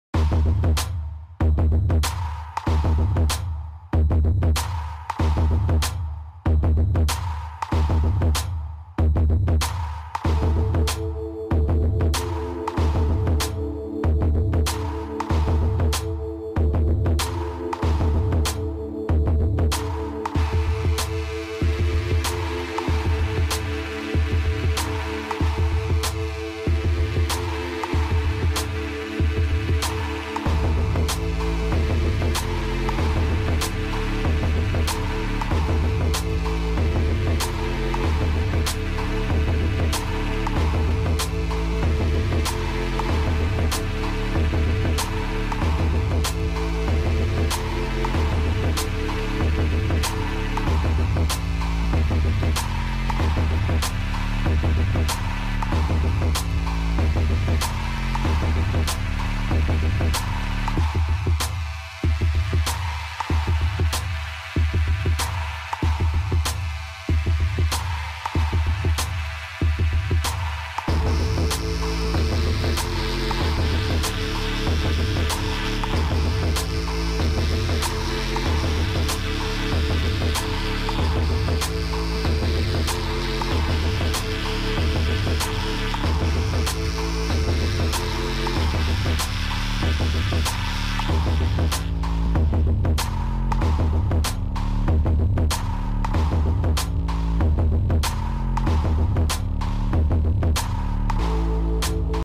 Phantom 3 Standard: Basketball Court Visit
Flew around the neighborhood,  stopped by the basketball court.
Song from YouTube selection, I can't remember whom is by. Sorry.